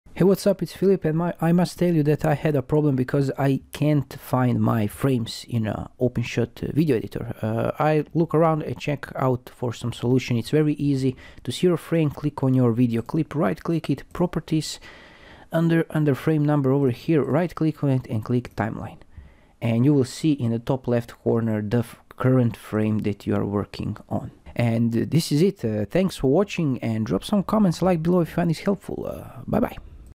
OpenShot Video Editor Tutorial: How to See Frames in Opensoht Video Editor
Tutorial on how to see frames in Opensoht video editor.